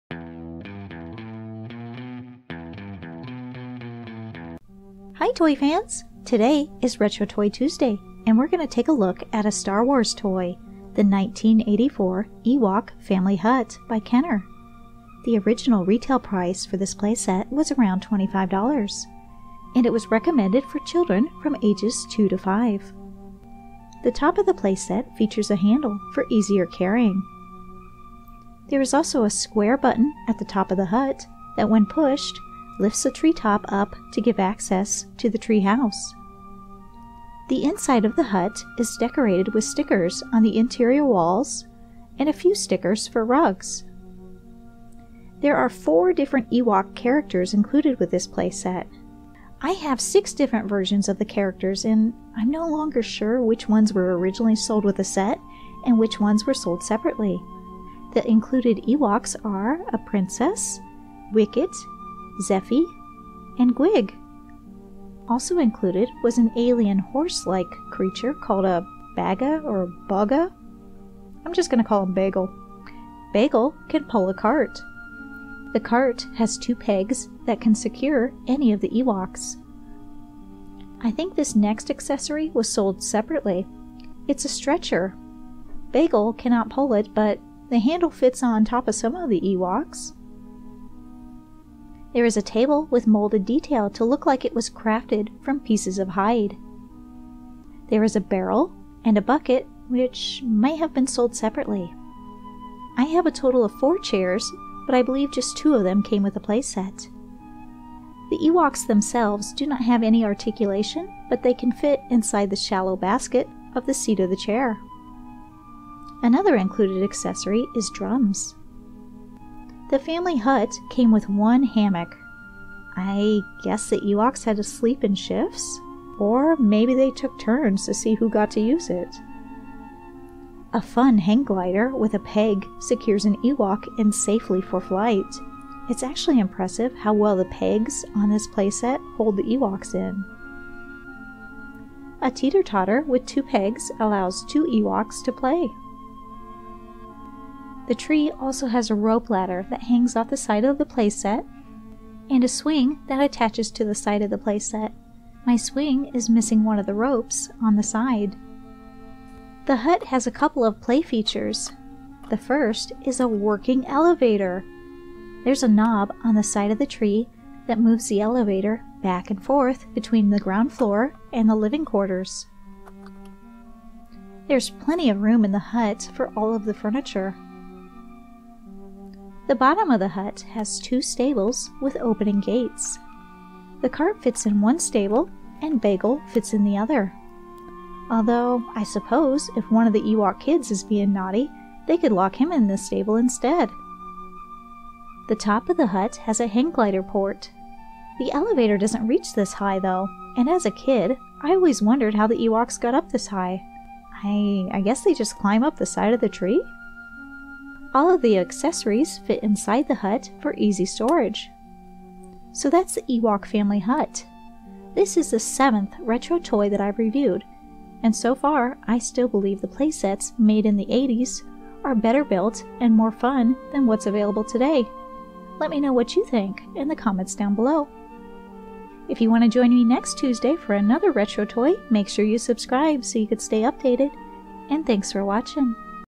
1984 Ewok Family Hut Playset Review
1984 Ewok Family Hut Playset Review
Today is retro toy Tuesday and we’re going to take a look at a Star Wars toy, the 1984 Ewok Family Hut by Kenner.  The original retail price for this play set was around $25 and it was recommended for children from 2 to 5 years old.

The top of the playset features a handle for easier carrying.  There is also a square button at the top of the hut that when pushed lifts the treetop up to give access to the tree house.

The inside of the tree house is decorated with stickers on the interior walls and a few sticker rugs.

There are 4 Ewok characters included with the playset.  The Ewoks included are Wicket, Princess Kneesas, Zephee, and Gwig.

Also included was an alien horse called a Baga.  The Baga can pull a cart.  The has two pegs that can secure any of the Ewoks.

There is a table with molded detail to look like it was crafted from pieces of hide.  There is a barrel and bucket which I think were sold separately.  I have a total of 4 chairs, but I believe two of them came with the playset.  The Ewoks do not have articulation, but they can fit inside the shallow basket of the seat of the chair.  Another accessory is the drums.

The family hut came with one hammock.  A fun hang glider with a peg secures an Ewok in safely for flight.  It’s actually impressive how well the pegs on this playset hold the Ewoks.

A teeter totter with pegs allows two Ewoks to play.

The tree also has a rope ladder that hangs off the side of the playset and a swing that attaches to the side of the playset.

The hut has a couple of play features.  The first is the working elevator.  There is a knob on the side of the tree that moves the elevator back and forth between the ground floor and the living quarters.

There is plenty of room in the hut for all of the furniture.

The bottom of the hut has two stables with opening gates.  The cart fits in one stable and the Baga fits in the other.

The top of the hut has a hang glider port.

All of the accessories fit inside the hut for easy storage.

If you enjoyed this review, you may also enjoy my review of the Pee-wee’s Playhouse Playset